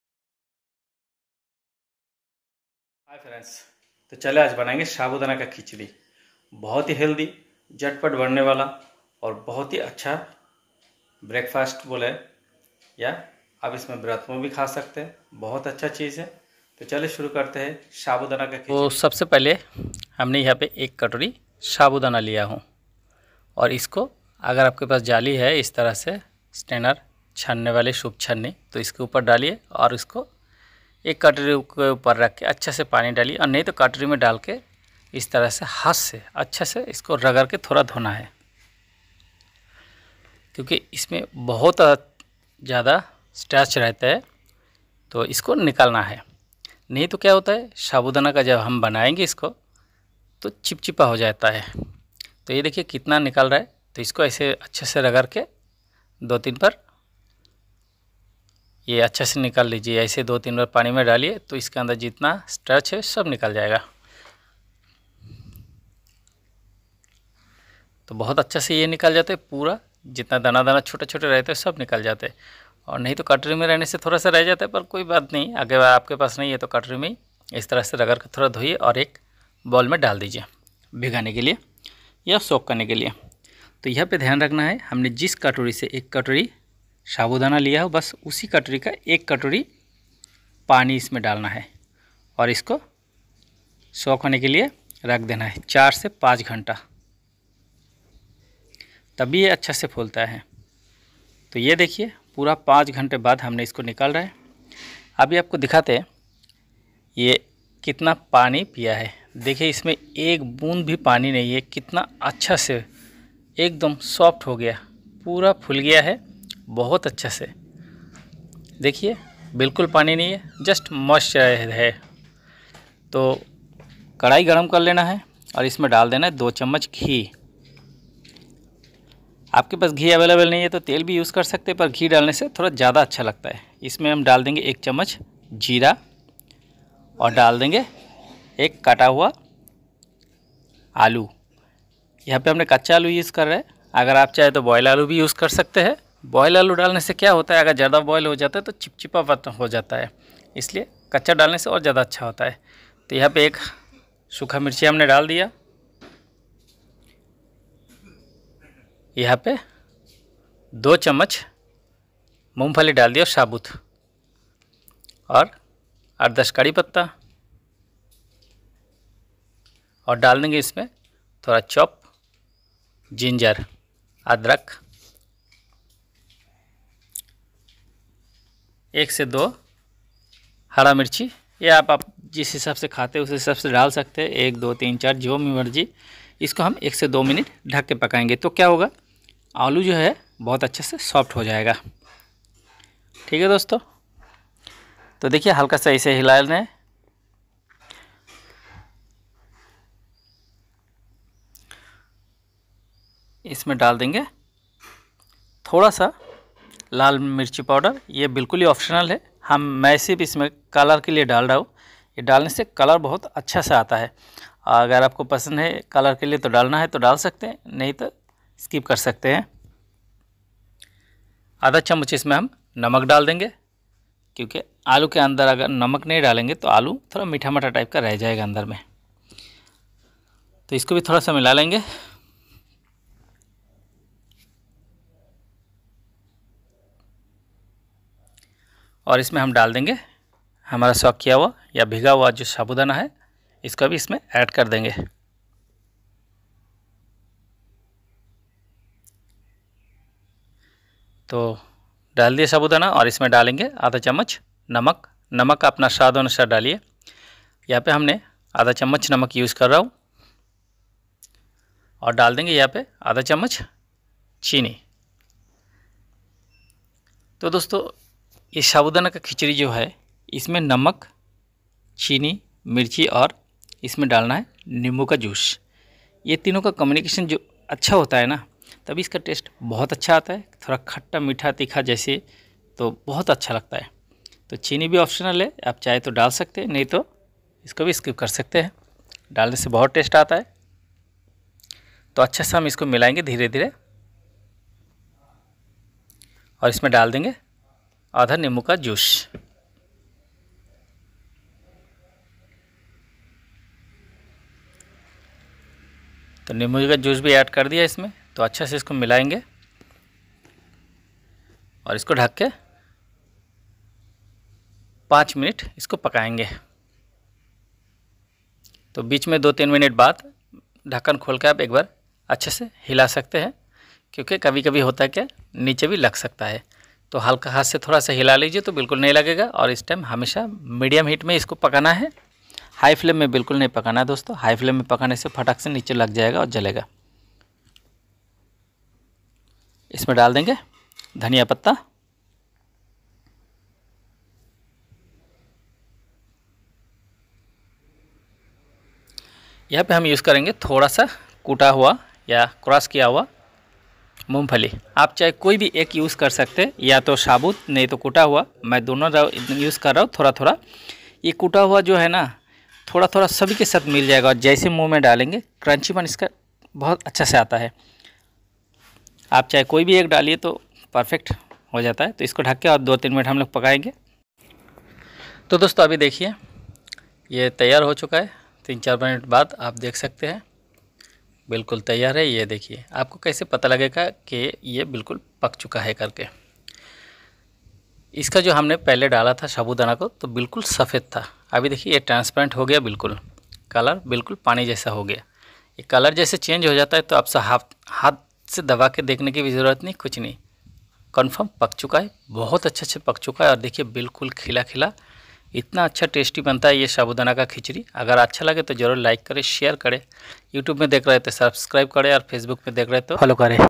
Sabudaneki Khichdi / Sabudana ke Khichidi / Homemade Sabudana ke Khichdi
Healthy breakfast Lajabab  Test Sabudane ke Khichdi,  Make it so easy in your kitchen.

                 Ingredients
1 Cup Sago (साबूदाना)
1 Cup Water for Soak (पानी)
2-3  tsp Ghee
1 tsp Cumin (जिरा)
1 Potato (आलु)
2 tbsp Peanuts (मूंगफली)
7-8  Curry leaves (कारिपाता)
2-3 Green Chilli (हरामिर्च)
Salt to test (नमक स्वादानुसार)
Pinch of Chilli powder (मिर्चपाउडर)
1/2 tsp Sugar Optional (चिनि)
1 tsp Lemon juice (निम्बू जुस)
Coriander leaves (धनिया पाता)
Crossed Peanuts 1/2 tsp (पिता मूंगफली)